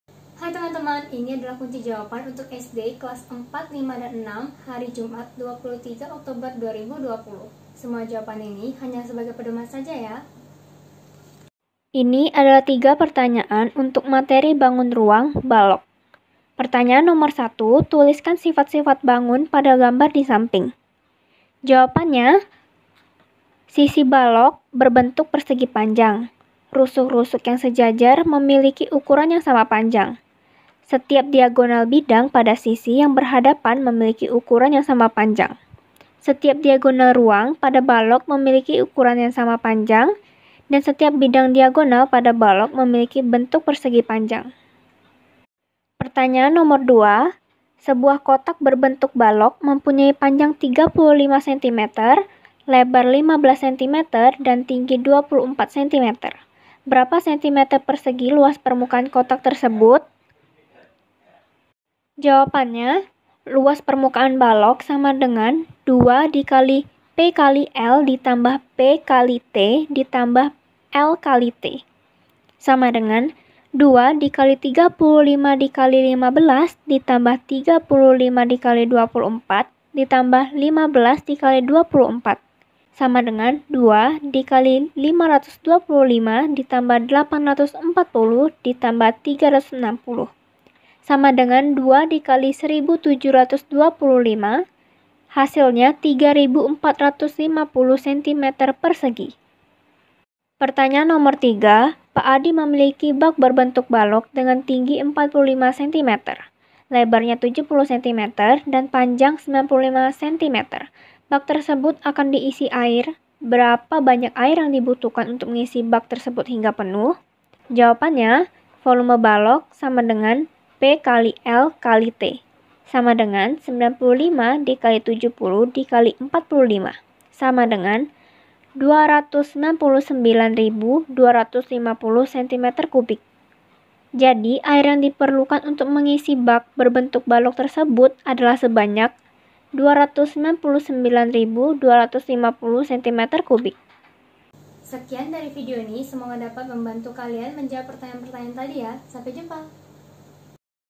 Kunci Jawaban TVRI Kelas 4-5-6 SD Jumat 23 Oktober 2020
Soal dan Kunci Jawaban TVRI Hari Ini Kelas 4-6 SD Jumat 23 Oktober 2020 materi Bangun Ruang (Balok)